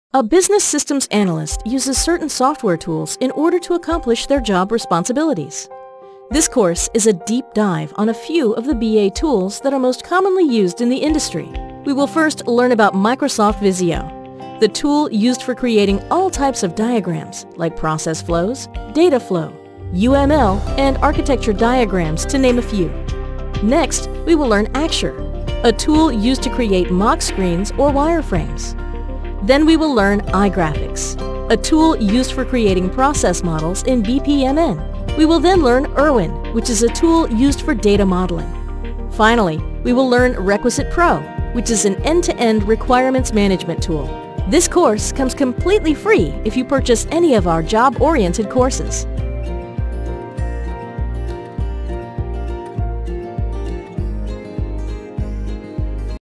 Business systems anakysis tools intro - Kuebiko
Introduction to software tools used by a business systems analyst in his/her everyday job. This course teaches details of Microsoft VISIO, Axure for wireframes, iGrafx for UI, ERWIN for data modeling and Requisite Pro for requirements management.